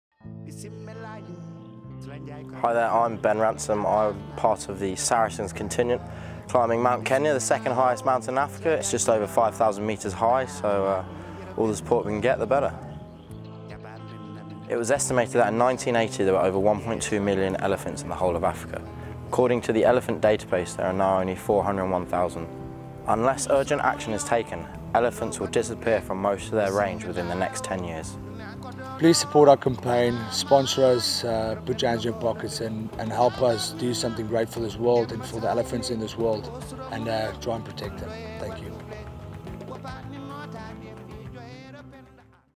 Ben Ransom's Climb for Giants: Please Support our Campaign
100,000 African elephants were killed by poachers between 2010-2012. That's equivalent to almost 100 African elephants killed every day or one killed every 15 minutes.

We must TRYHarder to combat the elephant poaching crisis and help MakeSpaceForGiants.

Please sponsor Namibian Captain and rugby legend, Jacques Burger, and Saracens players Mike Ellery, Rhys Gill, Jim Hamilton, Nils Mordt and Ben Ransom on an extraordinary adventure in aid of Space for Giants.

Follow us at #‎TRYHarder

Space for Giants would like to thank Saracens, the players and all those kindly sponsoring them for your kind support helping us to protect elephants and the landscapes they depend on, forever.